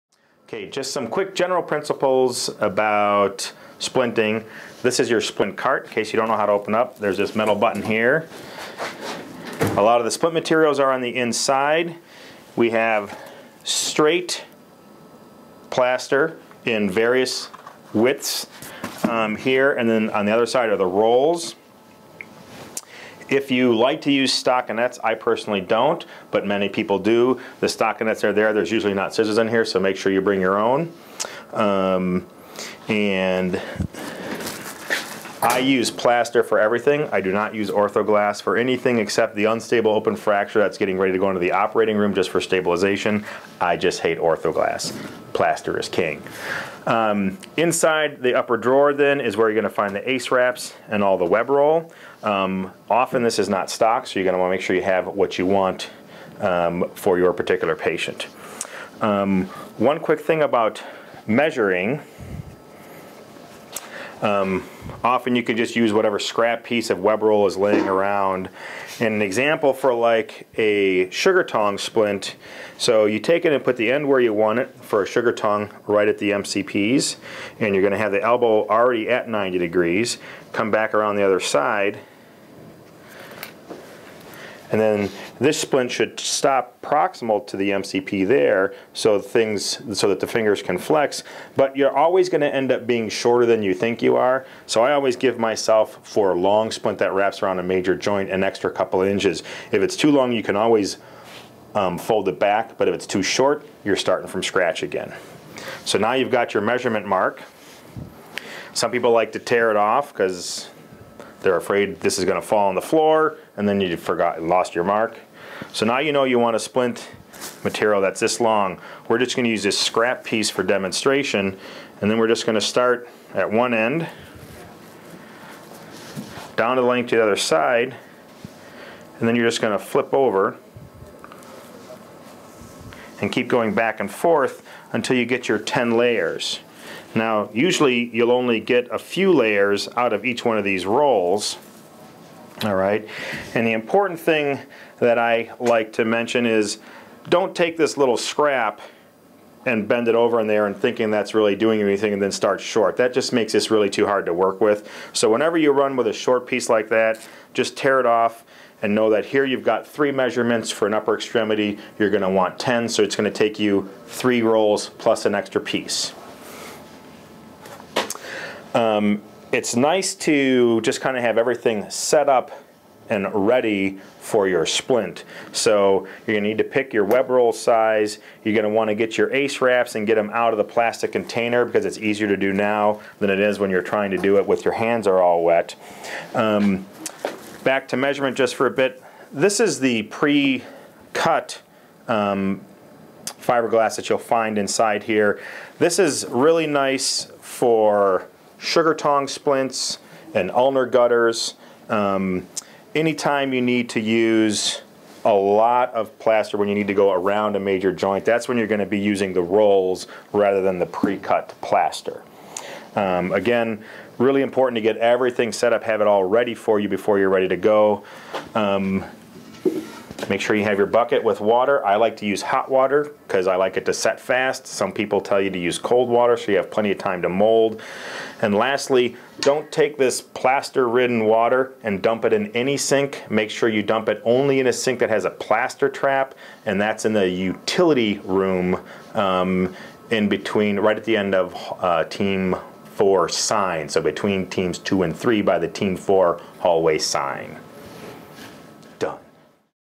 Splint Workshop 1 - Equipment: by ER Physician at Froedtert & The Medical College of Wisconsin
Splint Workshop 1 - Equipment: Dr Daniel Worman, Associate Professor of Emergency Medicine at the Medical College of Wisconsin, demonstrates the tools and equipment for applying splints.